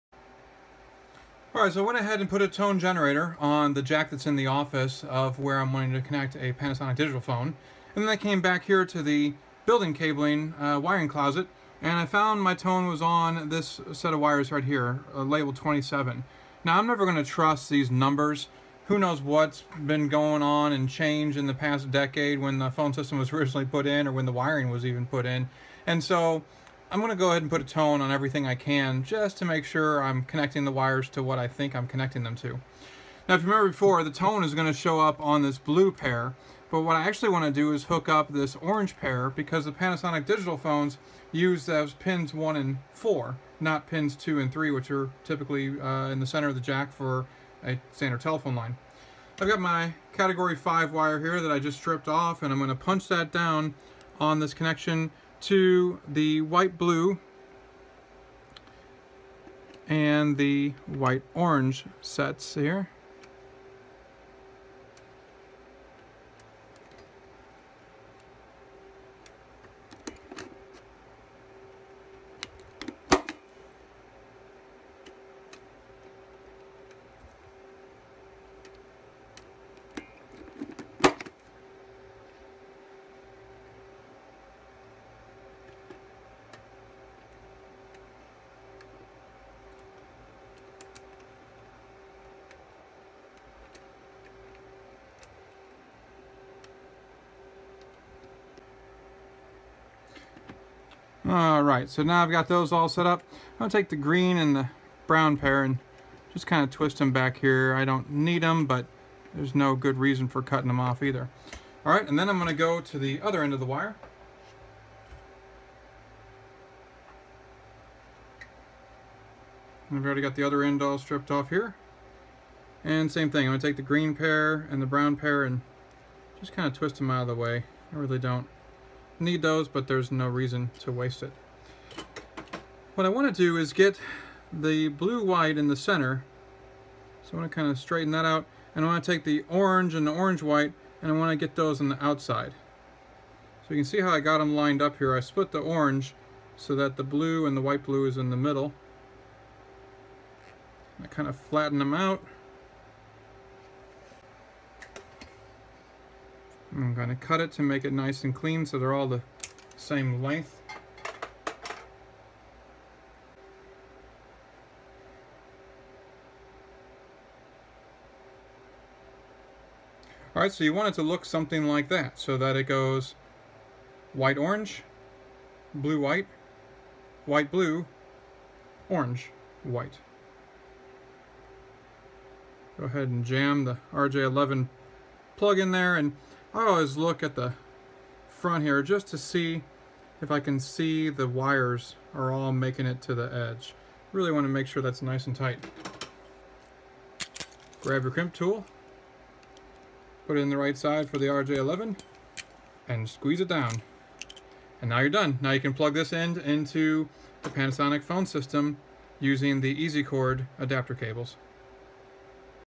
Installer Series - 05 Telephone Block Wiring and Troubleshooting
Installer Series archive video was originally created for only certified Panasonic digital hybrid IP phone system installer installation and troubleshooting of a pre-programmed system.  Use at your own risk.

Do you have an old Panasonic or another digital phone system you are ready to upgrade to new, better technology?